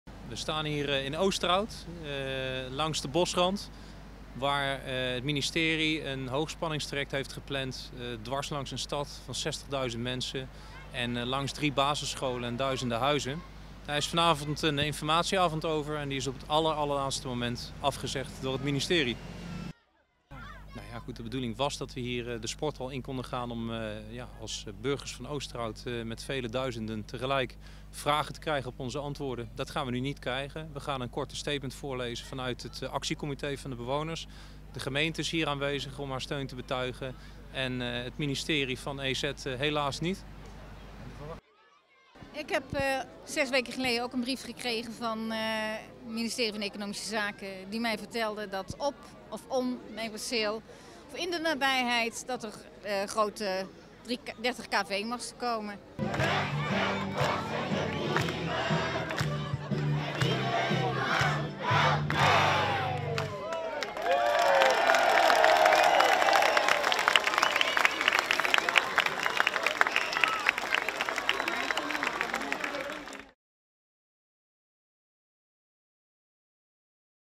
(VIDEO) 30-09-2014 Duizend bewoners protesteren tegen komst hoogspanningsmasten
OOSTERHOUT - De protestavond van bewoners in Oosterhout tegen de toekomstige hoogspanningslijn is dinsdagavond massaal bezocht. Het actiecomité '380kV Oosterhout Nee' heeft ongeveer duizend mensen op de been gekregen.
De bewoners verzamelden zich om zeven uur bij sporthal de Oosterheide. Daar zongen driehonderd kinderen uit de wijk een protestlied en las het actiecomité een statement voor.

Het ministerie van Economische Zaken en netbeheerder TenneT zouden dinsdagavond ook aanwezig zijn. Om veiligheidsredenen zijn zij afgehaakt voor de geplande informatieavond.

Al weken onrust
Er is al weken onrust over een geplande hoogspanningslijn. Het ministerie was eigenlijk van plan om de nieuwe hoogspanningsleiding tussen Borssele en Tilburg via een noordelijke route te laten lopen.

Eind augustus kwam ineens het bericht dat toch is gekozen voor een andere optie en die loopt via bedrijventerrein Borchwerf in Roosendaal, langs onder meer Oudenbosch en Etten-Leur en via de zuidrand van de wijk Oosterheide in Oosterhout naar Tilburg.